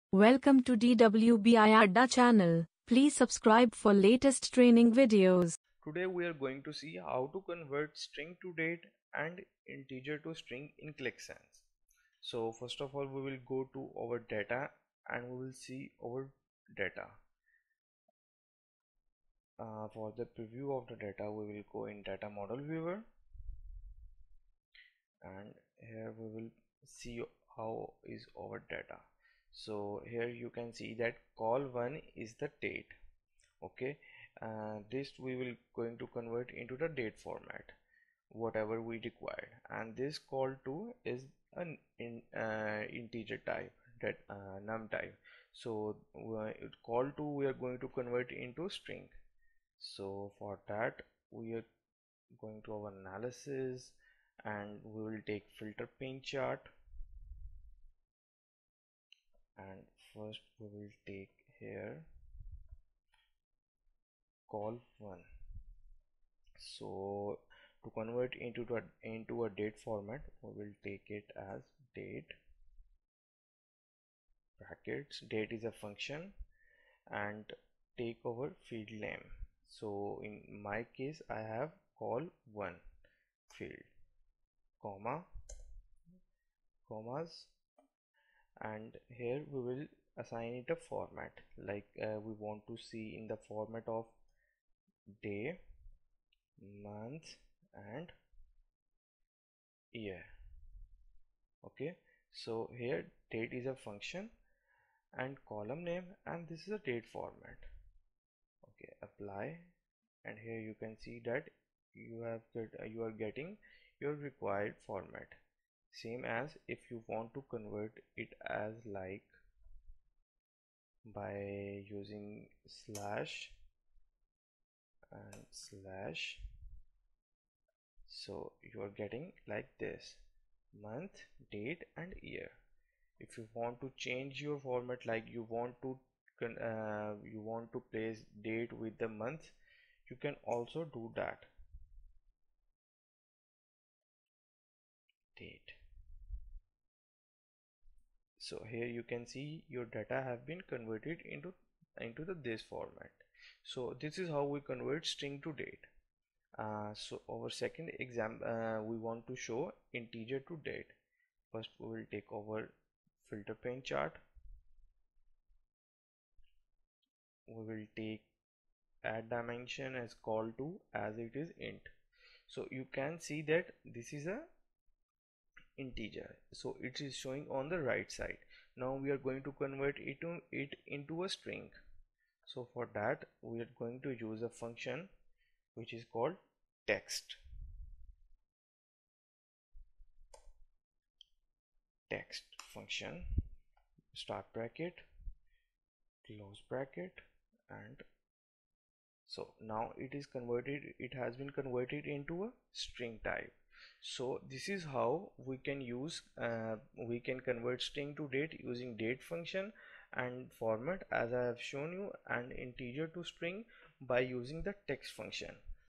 How to convert string to date int to string | Qliksense scenarios videos
Welcome to DWBIADDA's Qliksense scenarios and questions and answers tutorial, as part of this lecture we will see,How to convert string to date int to string